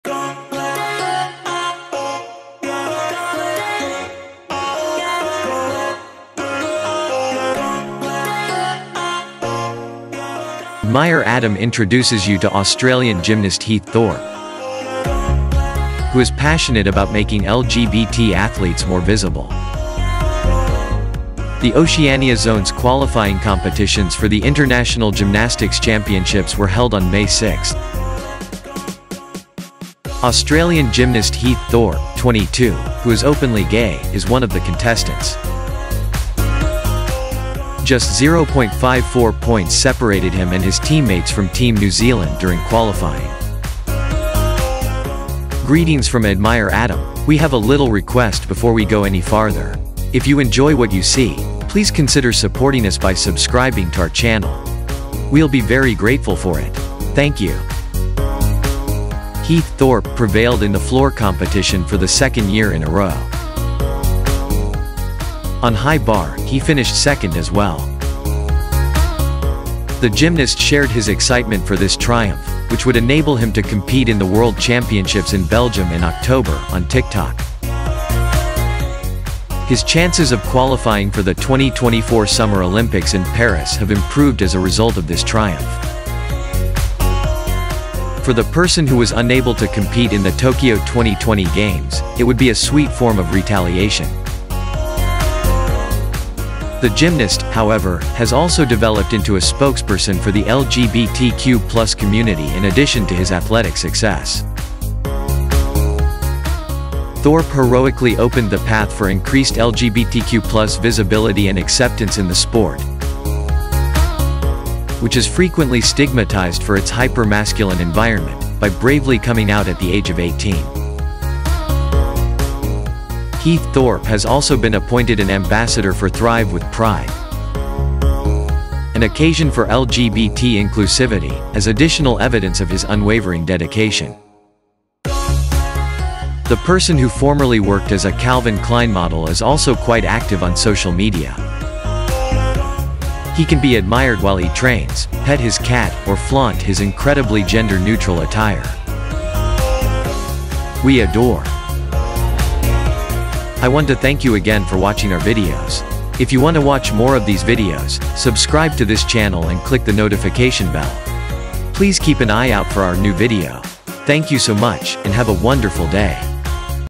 THRIVE FOR PRIDE WITH HEATH THORPE/Gymnast/Thrive for Pride Ambassador
Admire Adam introduces you to Australian gymnast Heath Thorpe, who is passionate about making LGBT athletes more visible. Admire Adam introduces you to Australian gymnast Heath Thorpe, who is passionate about making LGBT athletes more visible.

The Oceania zone's qualifying competitions for the international gymnastics championships were held on May 6. Australian gymnast Heath Thorpe, 22, who is openly gay, is one of the contestants. Just 0.54 points separated him and his teammates from Team New Zealand during qualifying.

Greetings from Admire Adam! We have a little request before we go any farther. We'll be very grateful for it!

Heath Thorpe prevailed in the floor competition for the second year in a row. On high bar, he finished second as well.

The gymnast shared his excitement for this triumph, which would enable him to compete in the World Championships in Belgium in October, on TikTok. His chances of qualifying for the 2024 Summer Olympics in Paris have improved as a result of this triumph.

For the person who was unable to compete in the Tokyo 2020 Games, it would be a sweet form of retaliation.

The gymnast, however, has also developed into a spokesperson for the LGBTQ+ community in addition to his athletic success. Thorpe heroically opened the path for increased LGBTQ+ visibility and acceptance in the sport, which is frequently stigmatized for its hyper-masculine environment, by bravely coming out at the age of 18.

Heath Thorpe has also been appointed an ambassador for Thrive with Pride, an occasion for LGBT inclusivity, as additional evidence of his unwavering dedication.

The person who formerly worked as a Calvin Klein model is also quite active on social media. he can be admired while he trains, pet his cat, or flaunt his incredibly gender-neutral attire. We adore!

I want to thank you again for watching our videos. Please keep an eye out for our new video! Thank you  so much, and have a wonderful day!